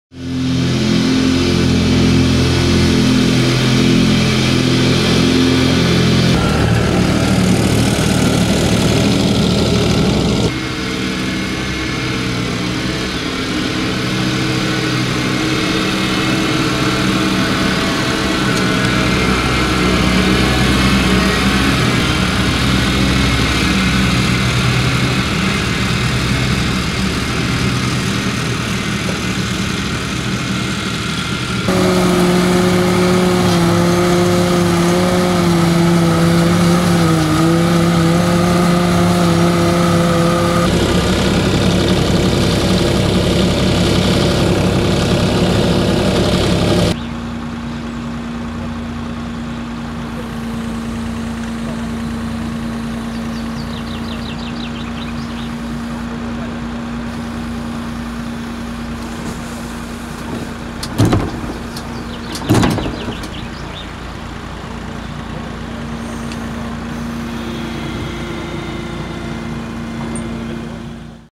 Avant Loader with Collecting Lawn Mower 1500mm attachment
The Avant Collecting Lawn Mower attachment for the Avant mini loader is designed for professional use with a 1500 mm cutting width and a 800 litre collector box. The high suction power makes it possible to collect tree leaves on lawns. The blades cut the grass, tree leaves etc. to a fine mulch which packs tightly into the collector box.

Collector box volume is designed to be large enough for professional use in larger areas. The box is emptied from the Avant mini loader driver seat with an electric switch and with the help of Avant mini loader boom the collector can be emptied into a container, skip etc

- Equipped with Optifloat™–system
- More cutting power for challenging mowing conditions
- Vacuum fan can be turned off and mower can be used as side discharging type
- Rounded edges mean less damage to trees, buildings etc.

For more info on Avant mini loaders and attachments in Australia